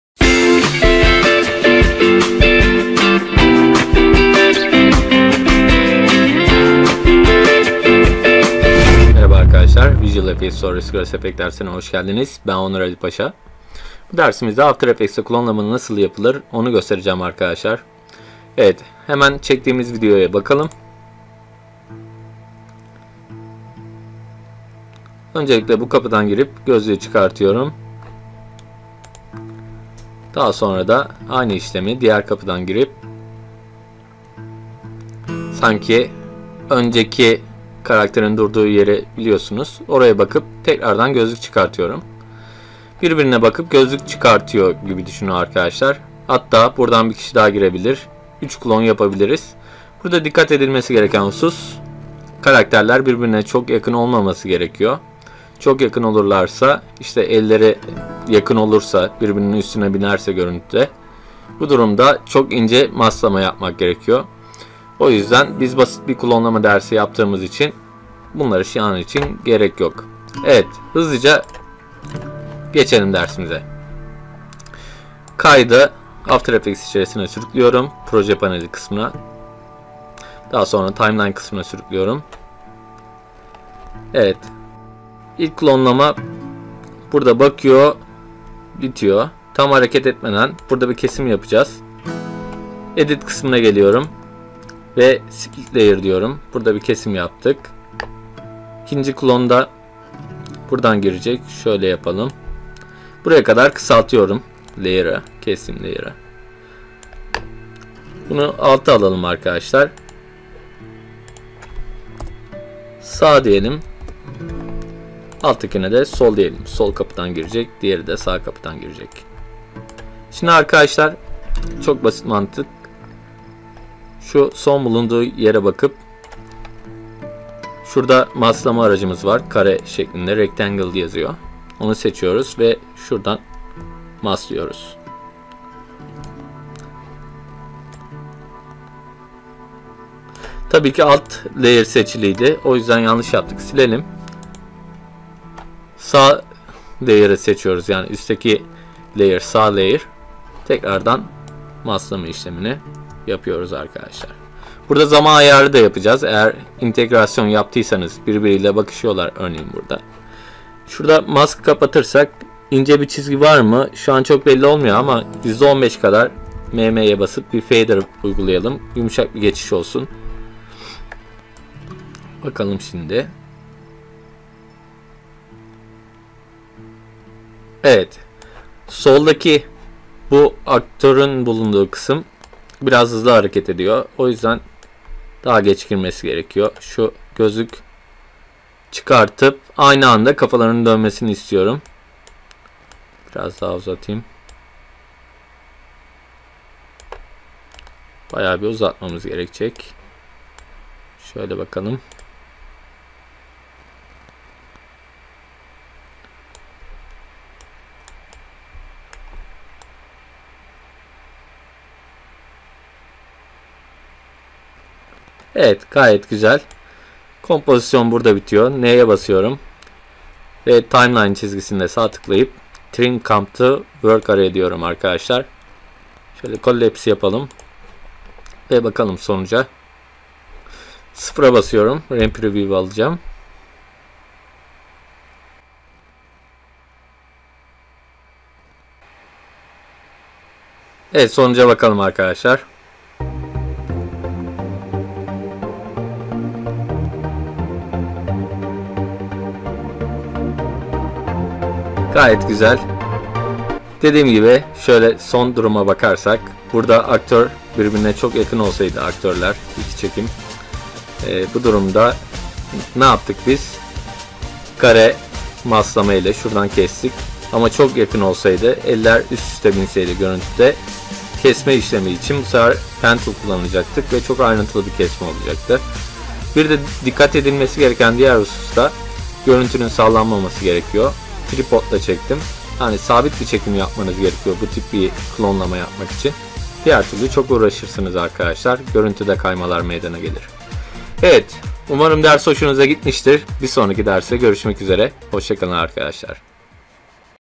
After Effects Dersleri | Klonlama - Adobe After Effects
Bu After Effects Dersimizde hızlı bir şekilde klonlama yapacağız. İyi seyirler.

After Effects Clone / Klon Yapımı